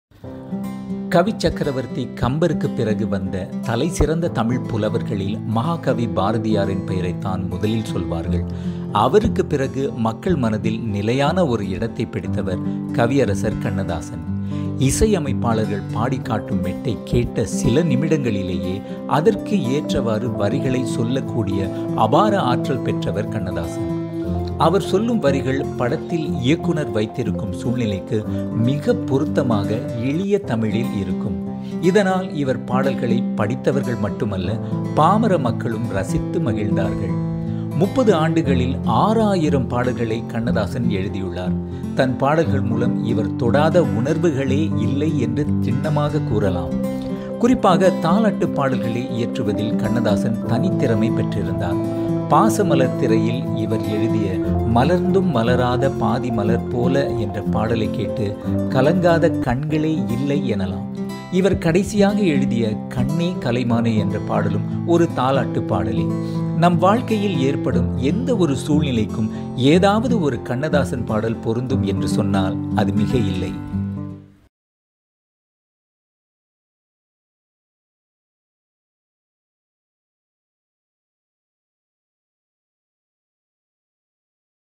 A brief biography of Poet Kannadasan by V Punithan
This is a brief biography of Poet Kannadasan by V Punithan, an ardent fan from Singapore.
Images used in the clip do not belong to me. They are the rightful properties of the creators themselves.